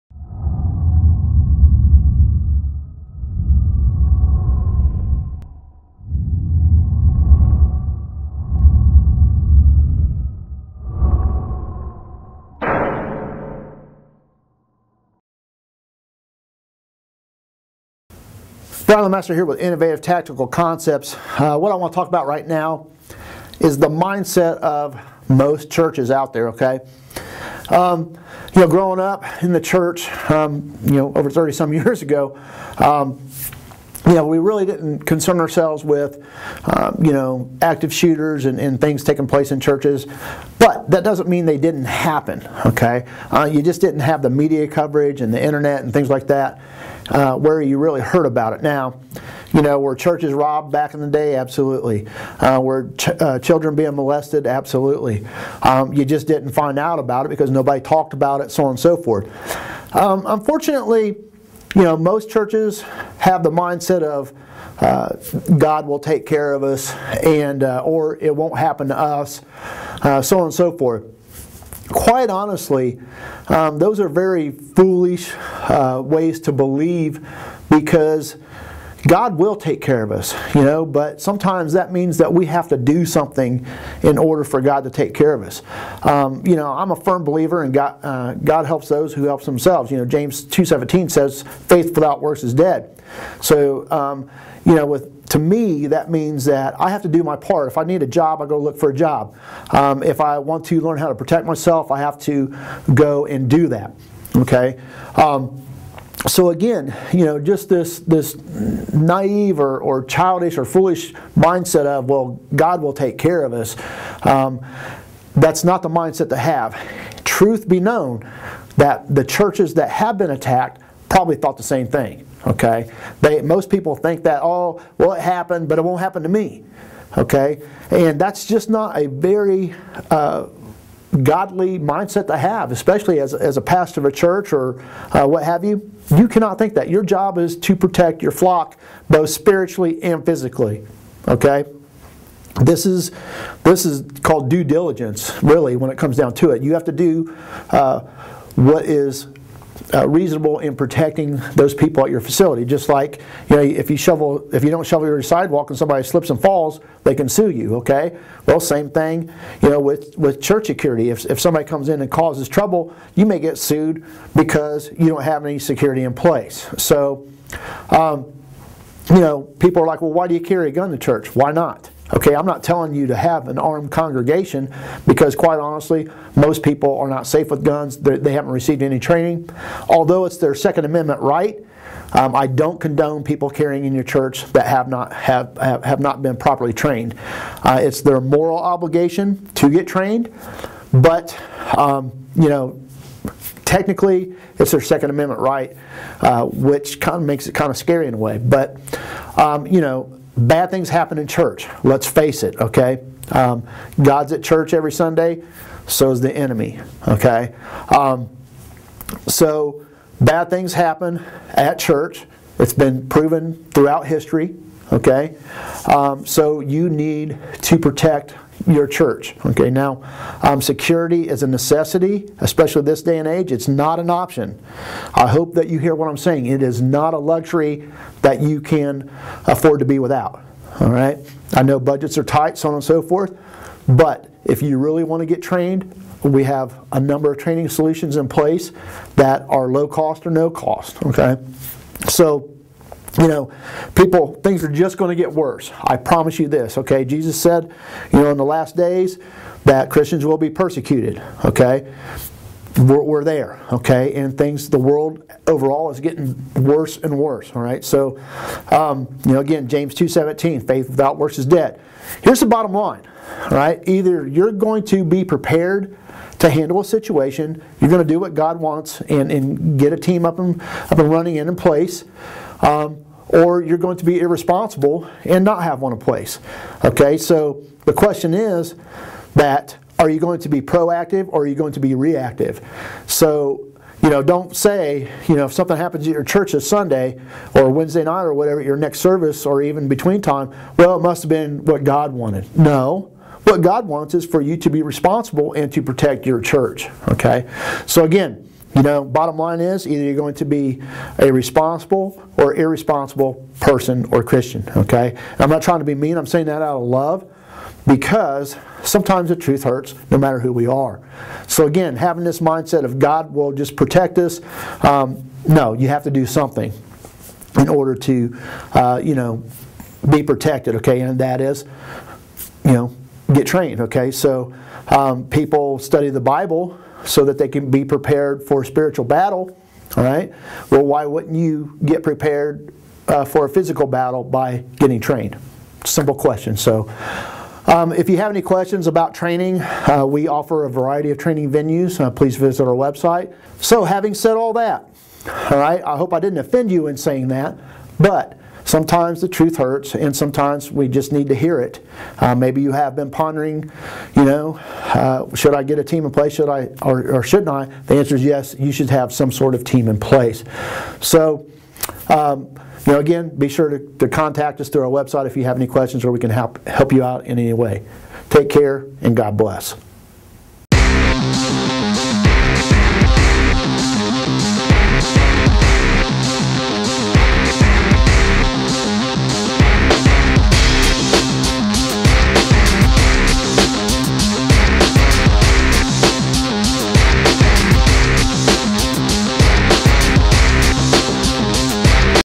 Church Security Team Mindset
In this video segment we will discuss the mindset of most churches regarding church security. Many church safety teams have the wrong mindset.